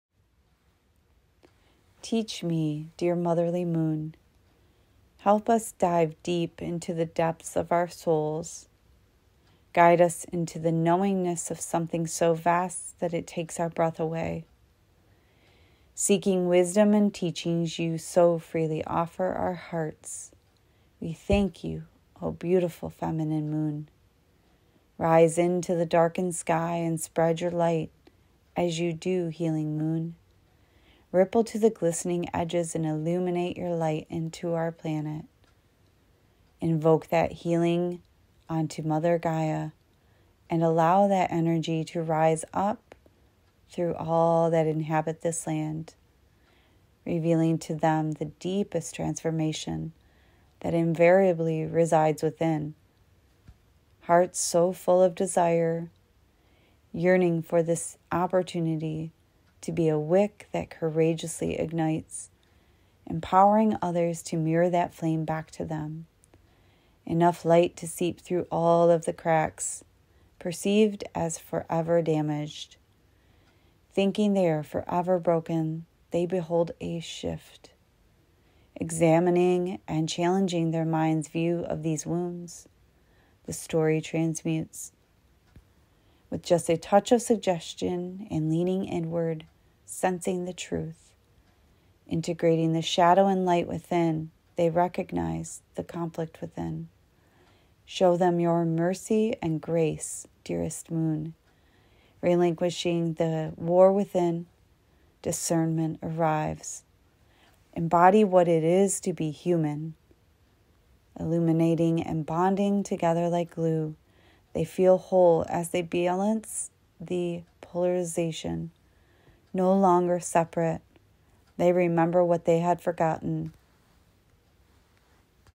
Dear Motherly Moon 🌝
The Full Moon in Leo 2022 occurs on Wednesday, February 16th at 11:56 AM EST. with the Sun in Aquarius.
Today is my 42nd birthday and embracing my soul’s journey by revealing the poet within. Starting to intentionally share these recordings of the writings I started almost two years ago.
Sitting quiet in solitude, Igniting the passionate writer within…I share these poems with you now. They have been sitting in my journals and in the notes section on my phone. I hope you enjoy them.
Seems like the perfect time to share a poem about the moon. Fill your heart this full moon, letting go and burning away what no longer serves you. Make room for new passion and excitement while remembering the ripple effect you have on the humans surrounding you. How would you like to proceed ? Shine like the Leo under this full moon and rise like the leader you are. Join together with others and continue walking to make a difference.